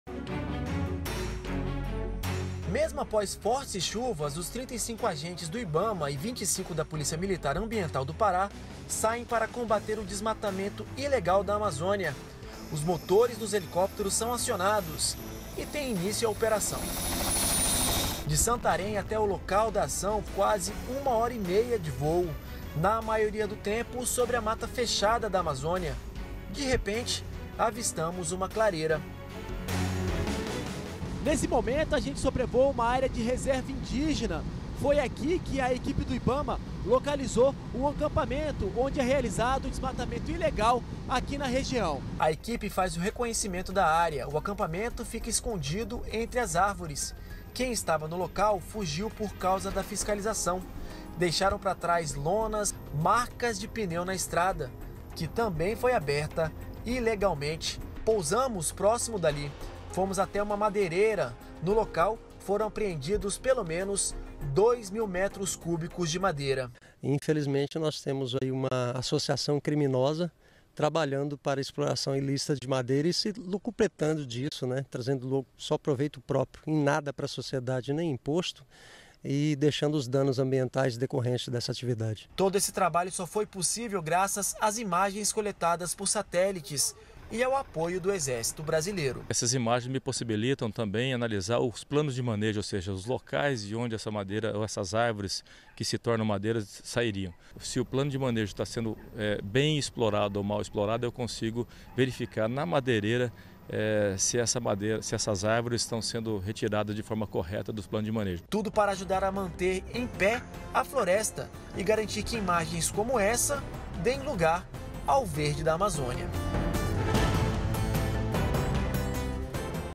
Ação do Ibama identifica desmatamento ilegal na floresta amazônica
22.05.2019 - Trabalho de fiscalização do Ibama identifica desmatamento ilegal na floresta amazônica, em Santarém, no Pará. Ação é possível graças a imagens coletadas por helicópteros e ao apoio do Exército.